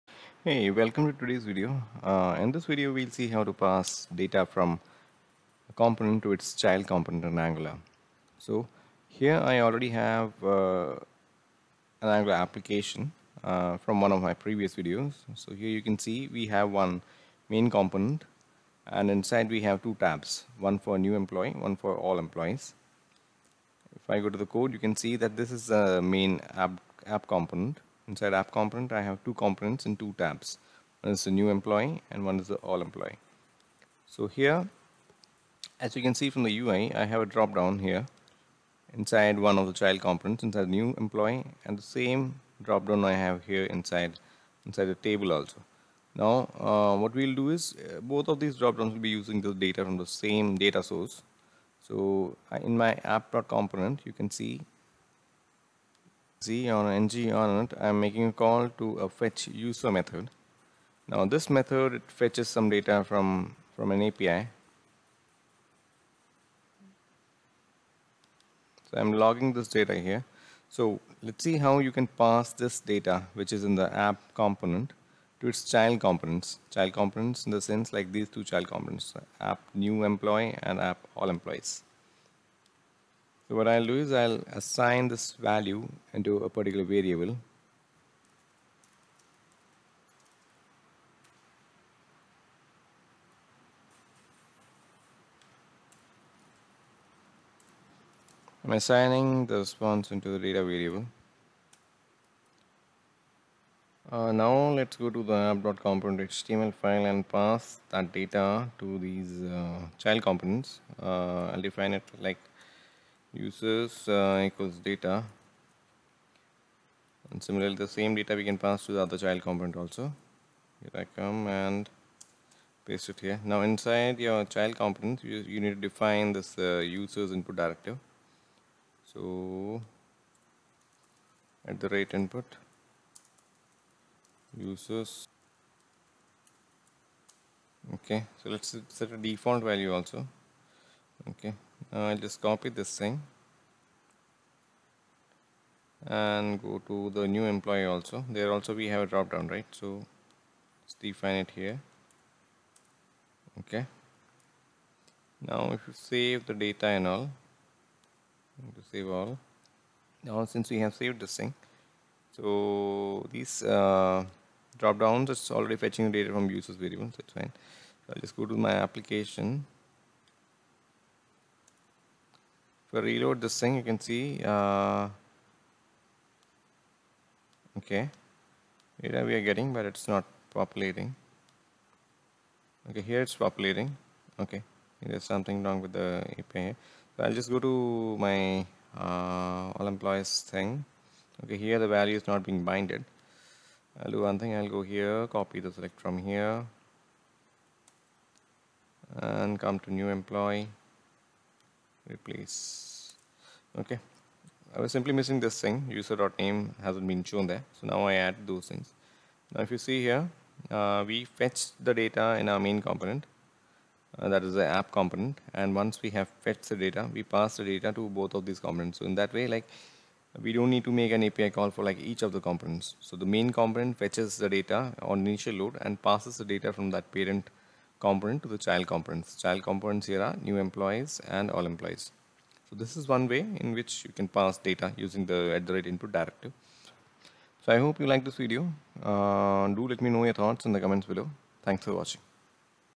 Angular : Pass Data From Parent To Child Components | @Input | With Source Code
In this video, you'll learn how to pass data from parent component to child component using @Input directive.